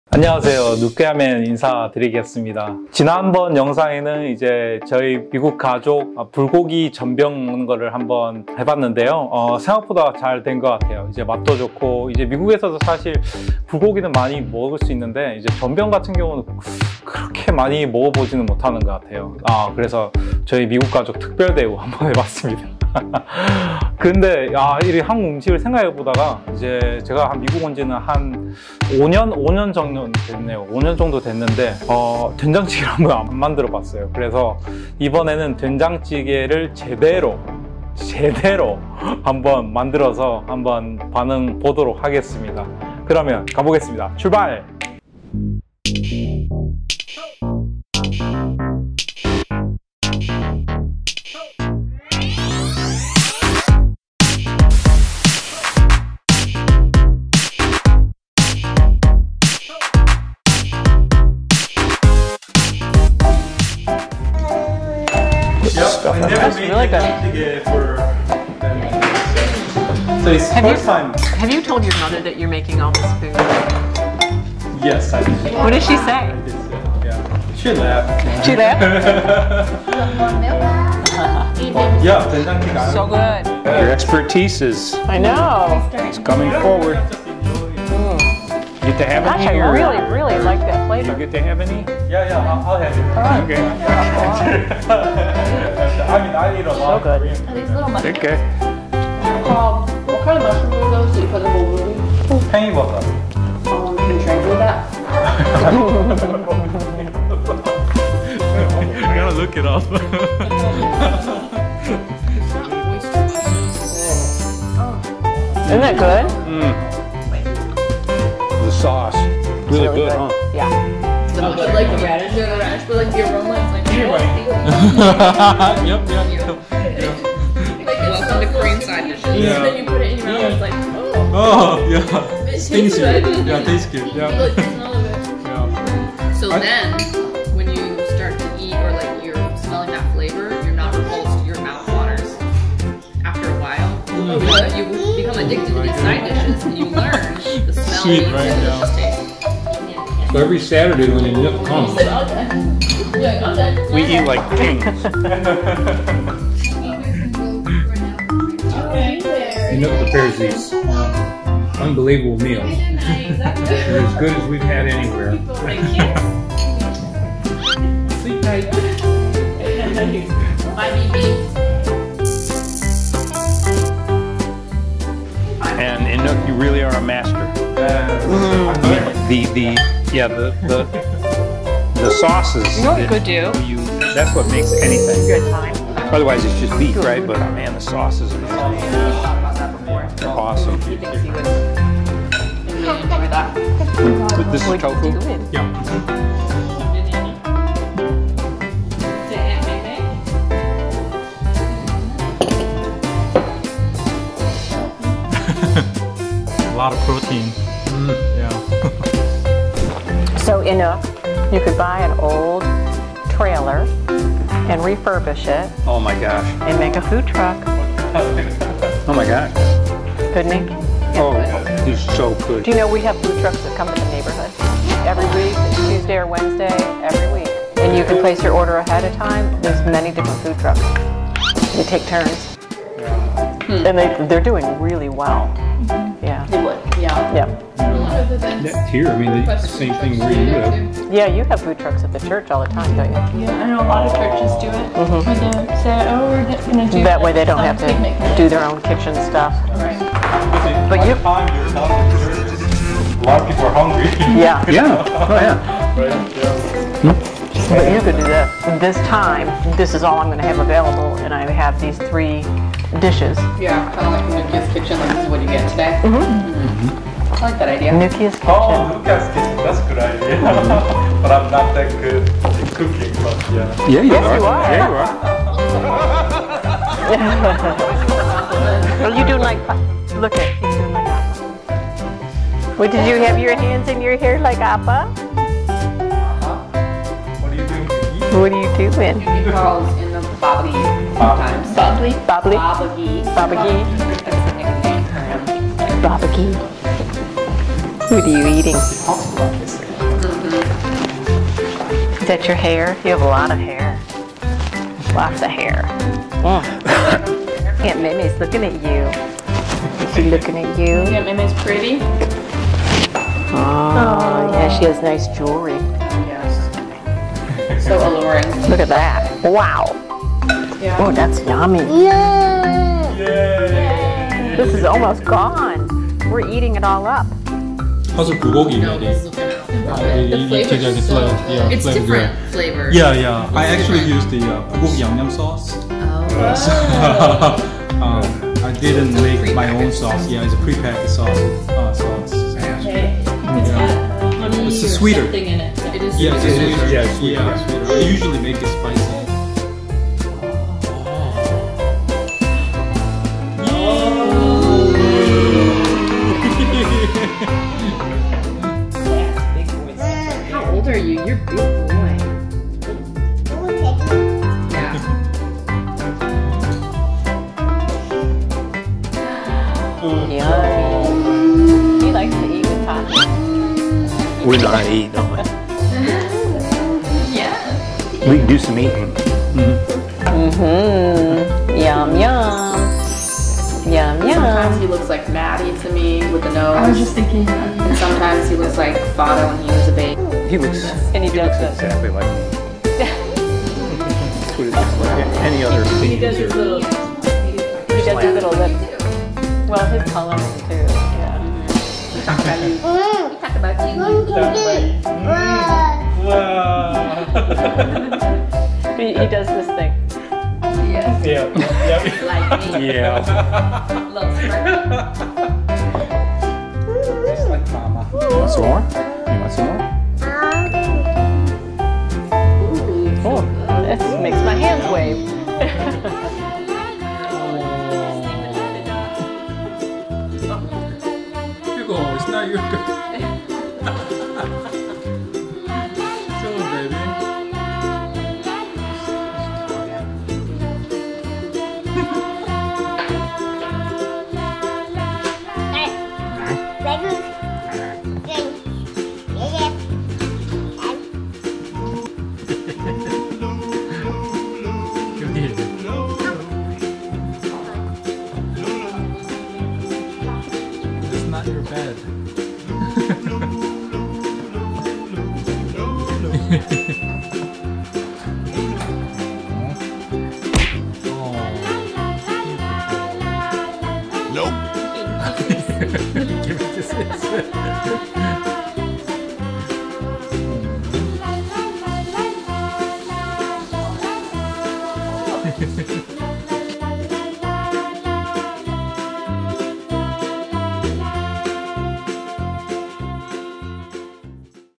[SUB] WHY KOREAN SON-IN-LAW? THIS IS WHY. (ft. soybean paste stew + bulgogi)
✅ 제목 Title: WHY KOREAN SON-IN-LAW? THIS IS WHY. (ft. soybean paste stew + bulgogi)┃ 🇺🇸 가족 된장과의 첫 만남┃ AMWF ┃국제커플

✅내용 Contents:
미국가족의 첫 된장 체험. 결혼한지 7년만에 미국가족이 처음으로 만든 된장찌개를 맛 보았습니다. 걸쭉하게 만든 된장 (두 번 끓인 업그레이드 된장)과 불고기 조합 괜찮네요. 요리 실력이 점점 늘어가는 것 같네요.

Been 7 years since we got married and I've never made soybean paste stew for my American family. Feel bad, so I decided to make soybean paste stew with bulgogi and  all tasted REAL good!
영상 하단에 있는 "Thanks (Super Thanks)" 버튼을 이용하여 현재 누꺄맨 컨텐츠 후원이 가능합니다!
Show us your support by clicking "Thanks (Super Thanks)" button below each video!